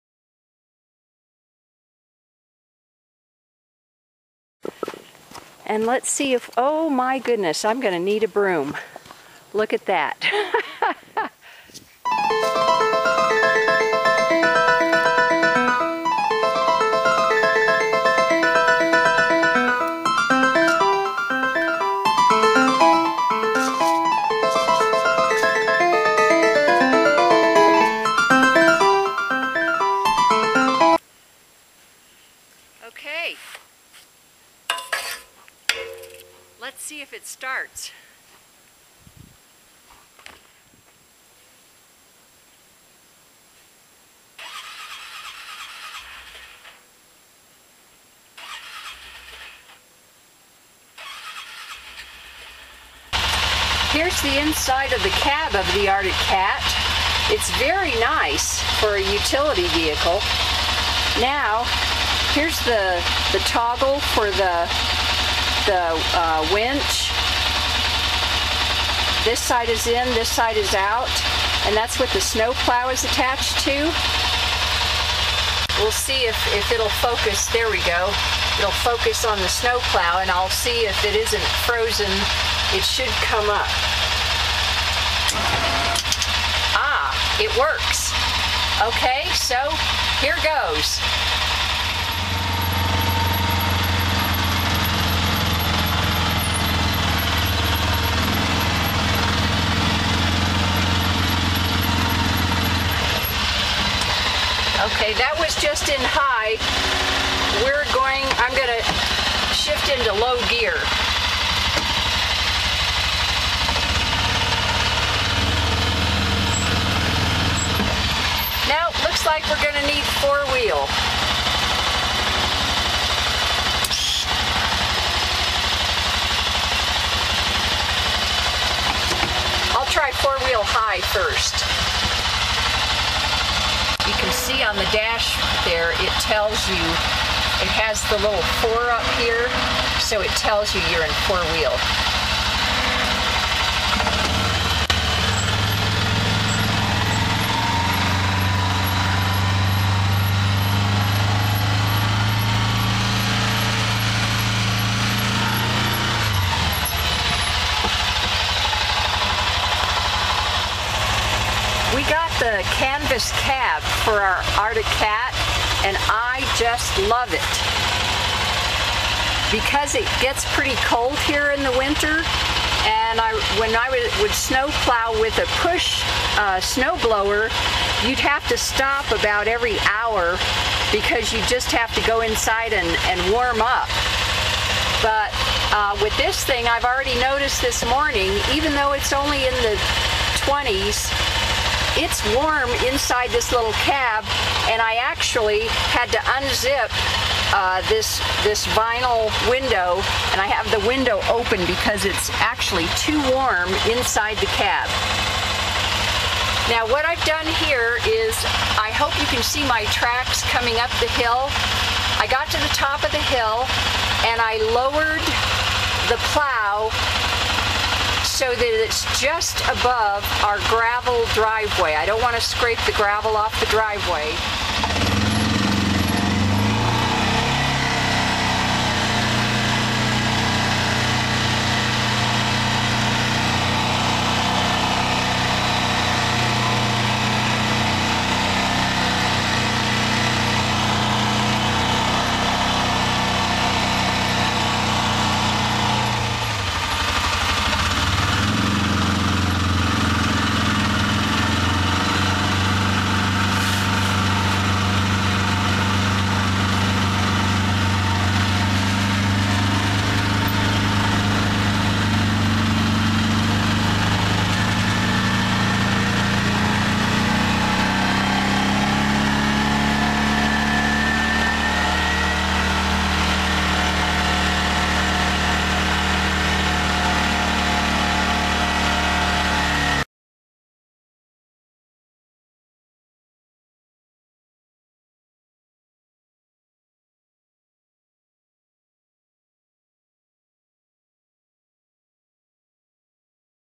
Snow in Colorado, Arctic Cat, Part 2 of 2
Plowing snow with a 2010 Arctic Cat Prowler 550.  The snow plow (from Arctic Cat dealer) is hooked at 4 points to a 3,000 lb winch installed on the Cat.

It has a canvas cab  with removeable doors, zippered vinyl windows & plexiglas windshield.

The "truck bed" has a manual dump lever.  Neat for unloading wood or cow manure for the garden.

We also got some landscaping attachments from Kolpin.  I'll do more video's showing how they work.  Some work well, others don't.